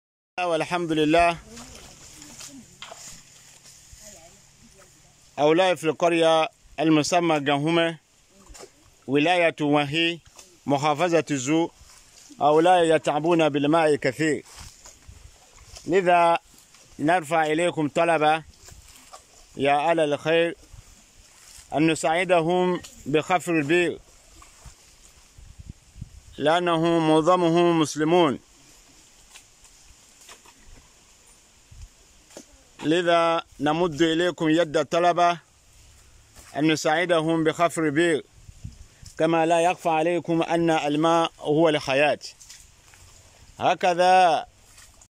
هذا هو حال الأهالي في دولة البنين 😢
لا حول و لا قوة الا بالله
كما تشاهدون هذا هو حال أهالي القرى في دولة البنين غرب أفريقيا ، بسبب ندرة المياه الصالحة للشرب.

📽️تصوير : الشيخ شريك كونكو